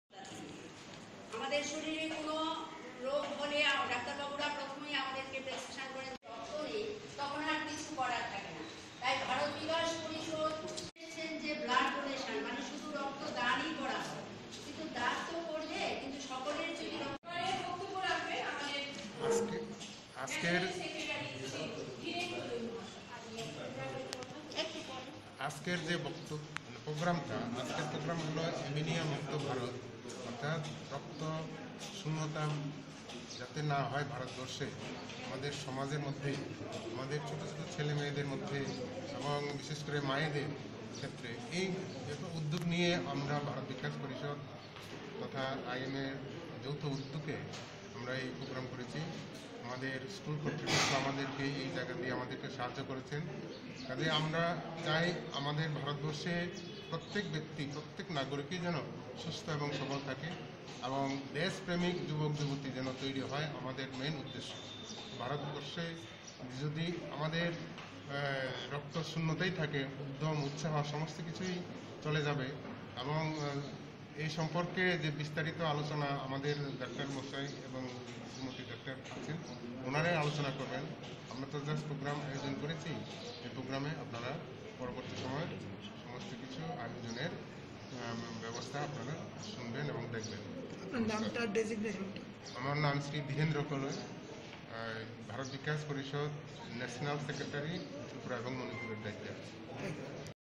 Bharat Vikas Parisad organized health camp.
Bharat Vikas Parisad organized health camp. TIWN Video Nov 22, 2020 in Agartala